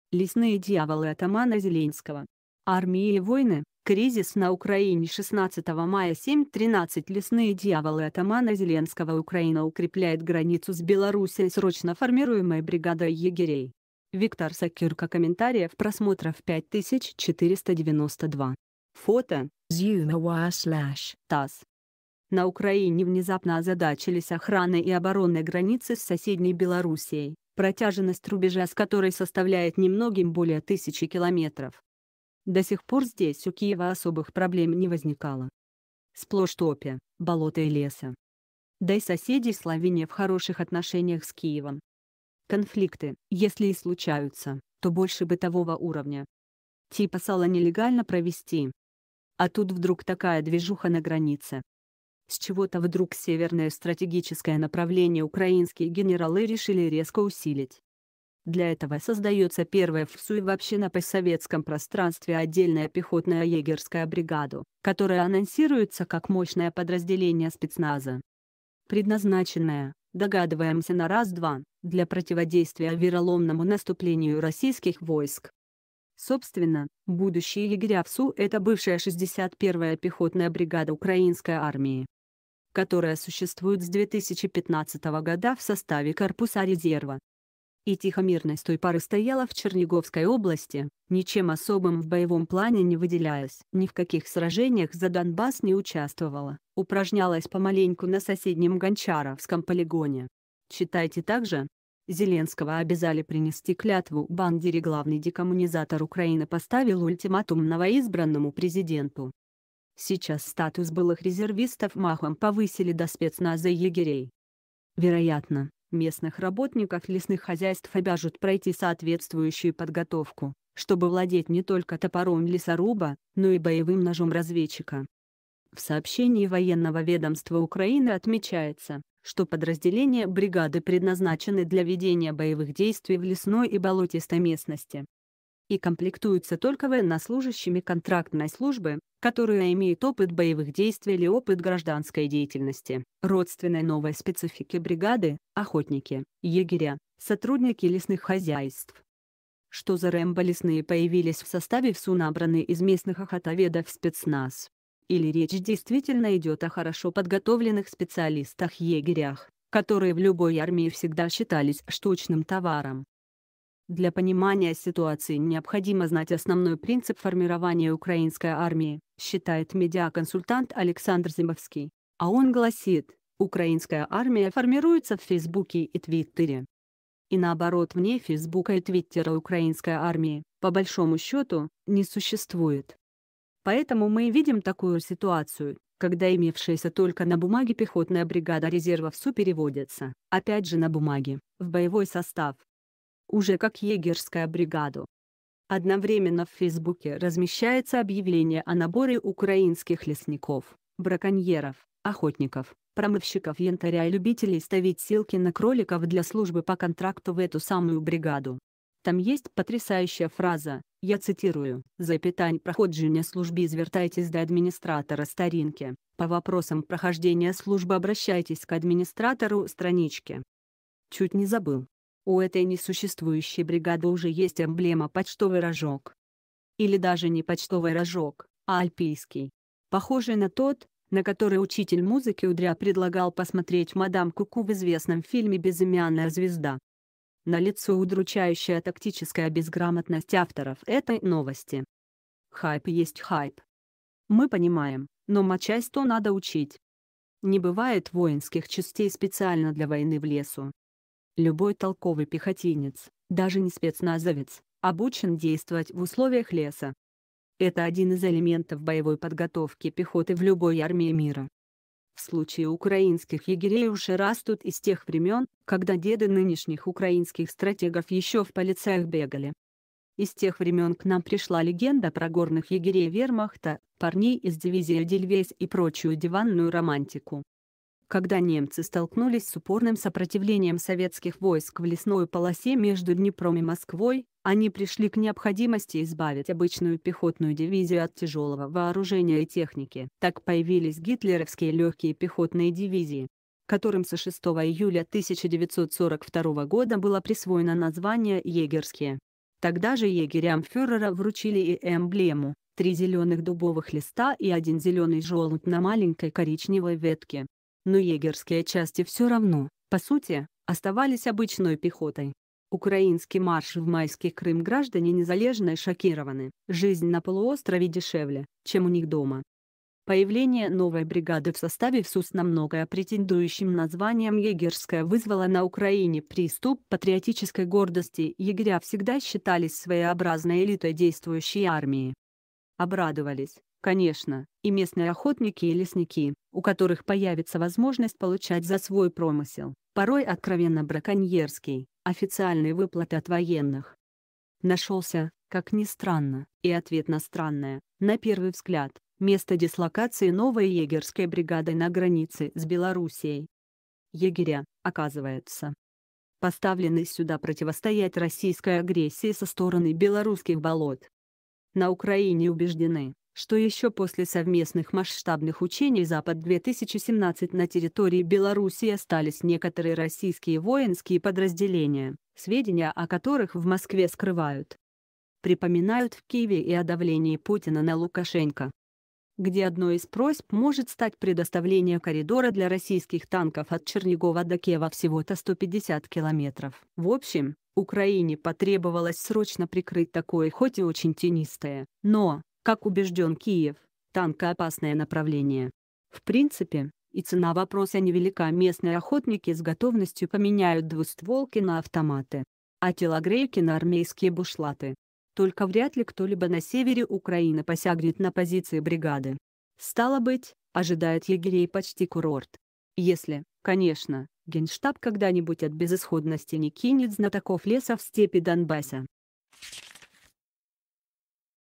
Лесные дьяволы атамана Зеленского
Лесные дьяволы атамана Зеленского Армии и войны Кризис на Украине 16 мая 07 13 Лесные дьяволы атамана Зеленского Украина укрепляет границу с Белоруссией срочно формируемой бригадой егерей Виктор Сокирко Комментариев Просмотров 5492 Фото ZUMA Wire ТАСС На Украине внезапно озадачились охраной и обороной границы с соседней Белоруссией, протяженность рубежа с которой составляет немногим более тысячи километров . До сих пор здесь у Киева особых проблем не возникало . Сплошь топи, б